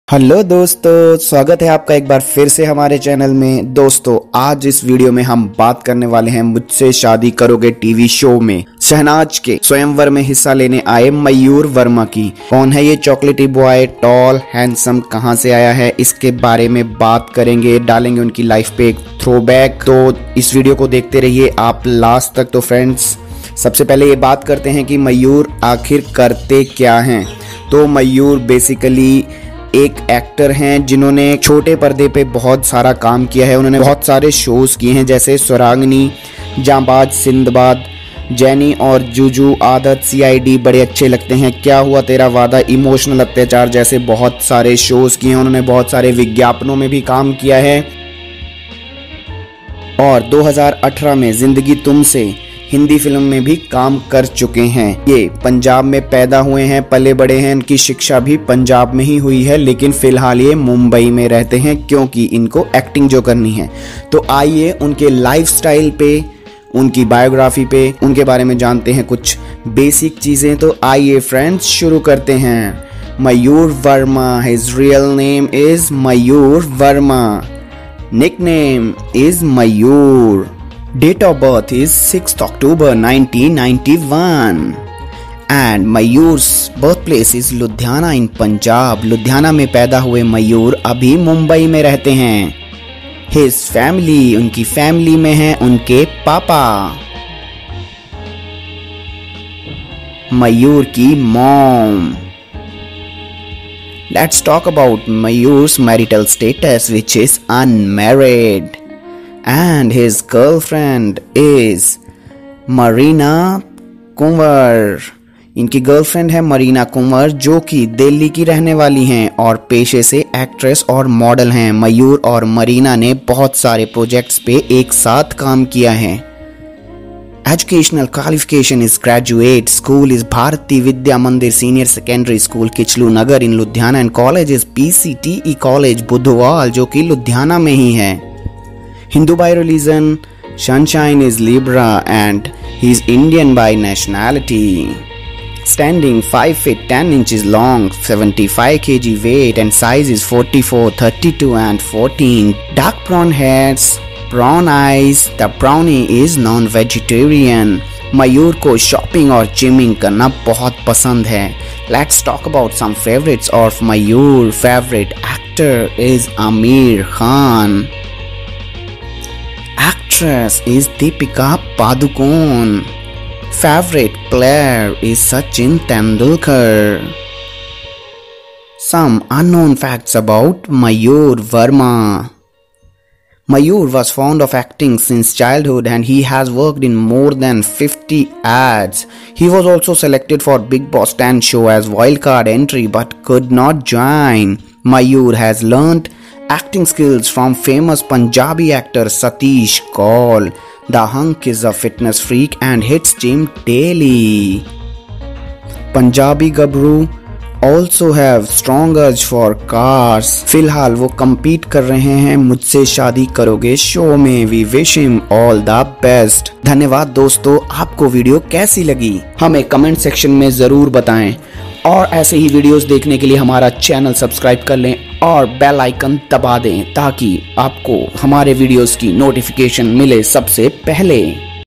Mayur Verma । मुझसे शादी करोगी Contestant l Lifestyle,Girlfriend,Family & Biography 2020
After Bigg Boss 13, Colors Tv has brought show Mujhse Shadi Karoge.
Mayur Verma is contestant in The show and competing for Sehnaaz Kaur Swayamwar. In this video we bring details about Mayur Verma Age, family, girlfriend, lifestyle and biography.